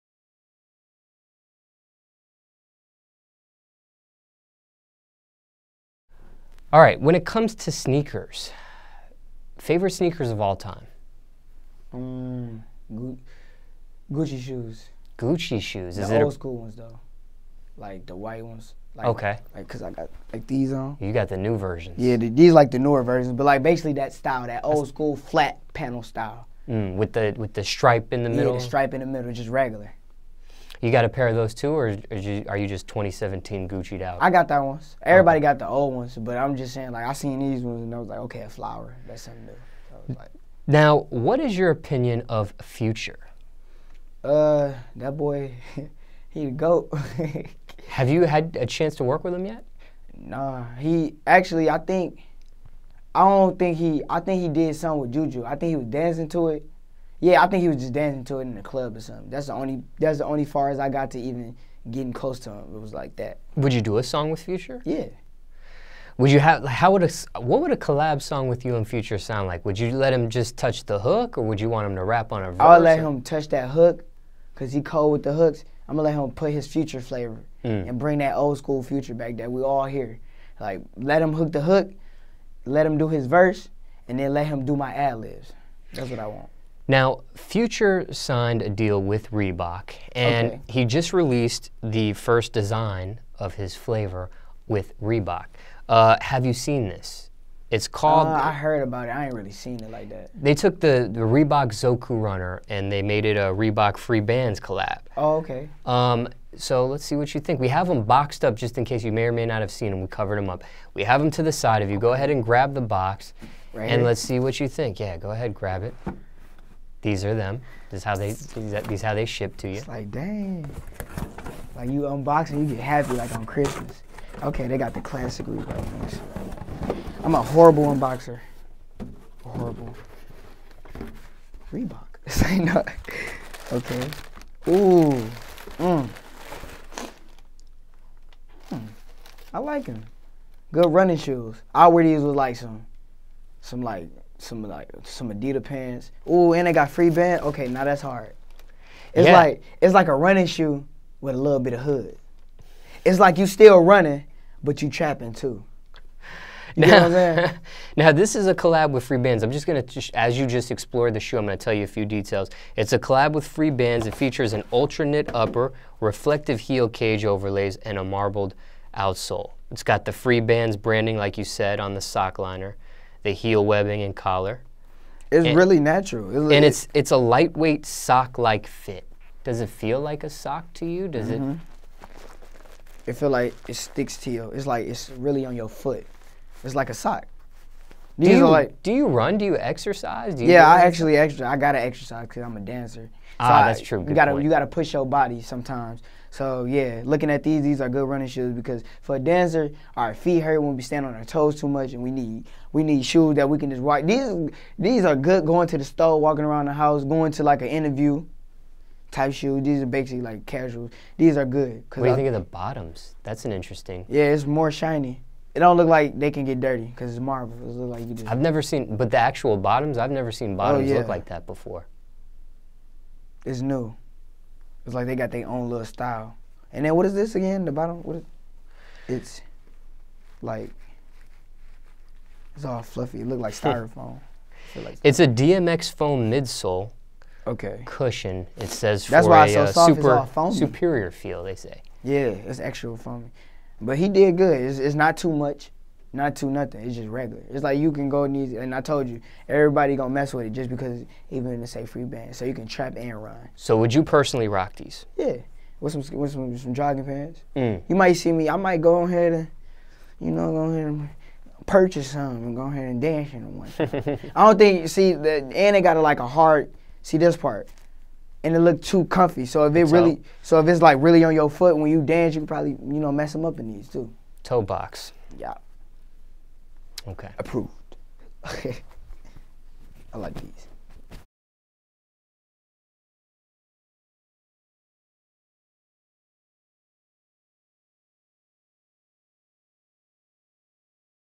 Reebok x Freebandz Zoku Runner Unboxing by Zay Hilfigerrr
Detroit rapper/dancer and Tha Lights Global member, Zay Hilfigerrr, sit down with DJ Smallz and unboxes Future's Reebok x Freebandz Zoku Runner in black/white.

Do you own a pair of Future's new sneaker?